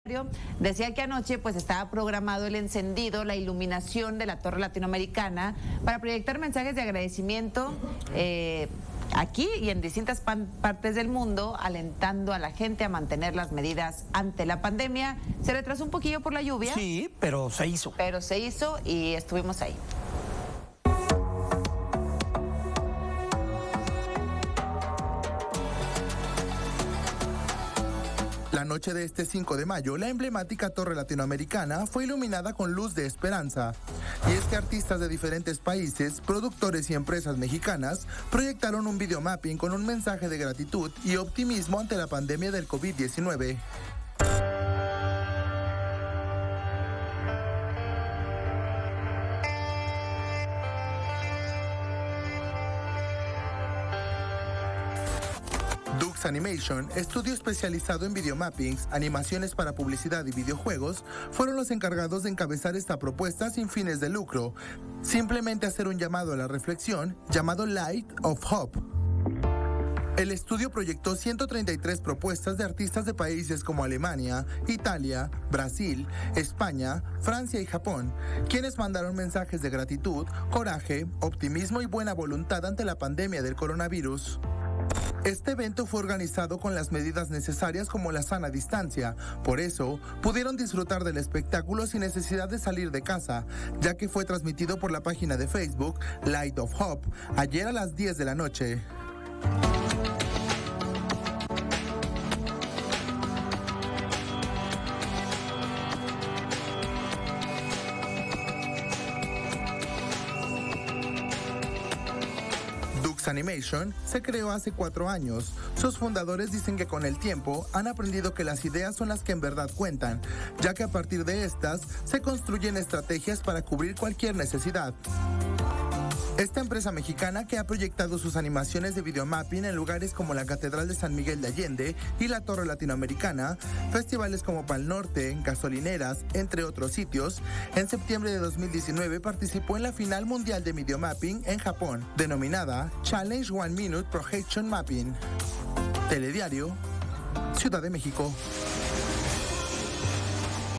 Torre Latino se ilumina con mensajes de esperanza por COVID-19
La Torre Latinoamericana se iluminó la noche de este martes con mensajes e imágenes de esperanza y agradecimiento ante la pandemia de COVID-19 (coronavirus). Los encargados de esta proyección sin fines de lucro fue la empresa Dux Animation.